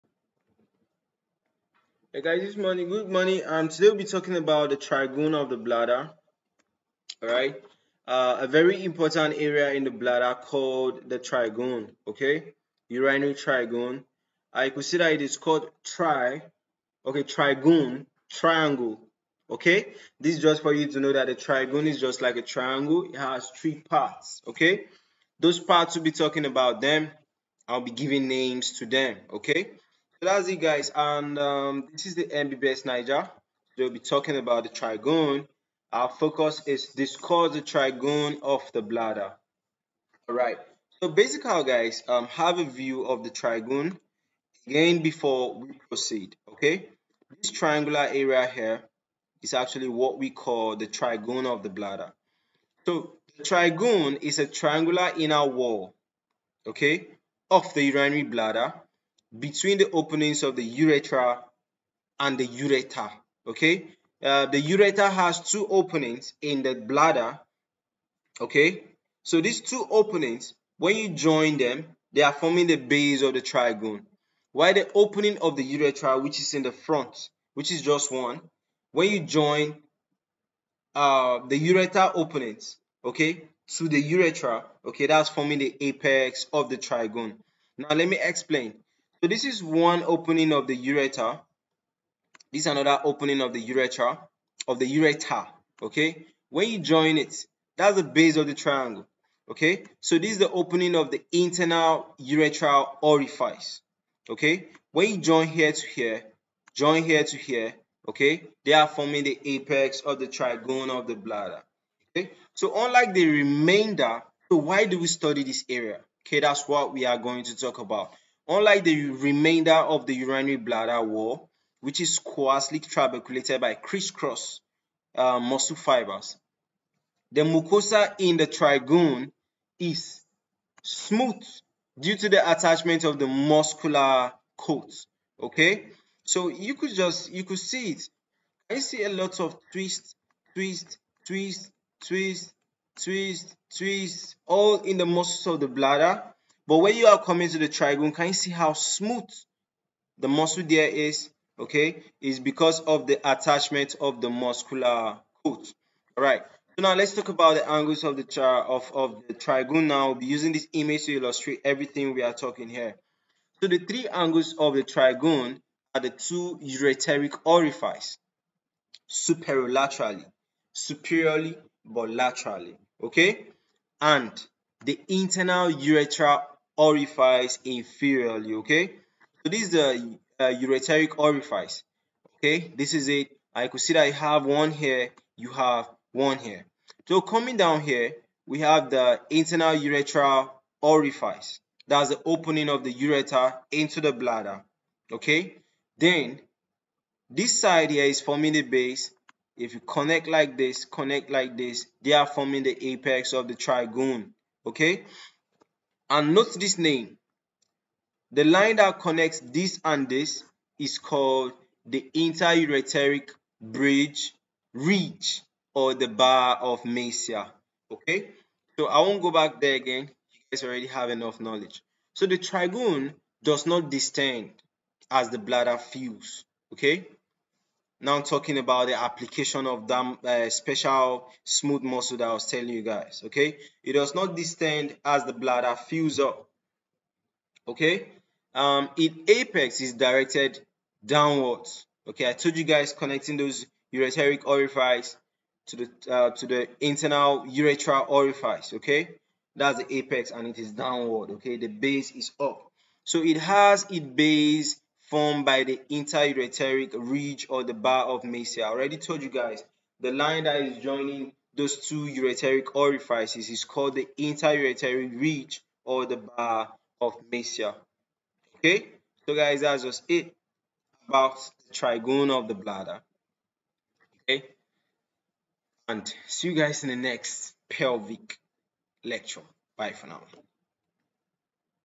The trigone of the bladder Anatomy; Ureteric orifice, internal urethral orifice and Bar of mercier
The trigone is the triangular inner wall (Of the base) of the urinary bladder between the 2 openings of the ureters and the urethra.

Unlike the remainder of the urinary bladder wall which is coarsely trabeculated by criss-cross muscle fibers, the mucosa in this area is smooth due to the attachment of the muscular coat.

The three angles of the trigone are the two ureteric orifices superolaterally and the internal urethra orifice inferiorly.

The trigone does not distend as the bladder fills

It has its apex directed downwards and forwards

Has its base formed by the interureteric ridge or bar of mercier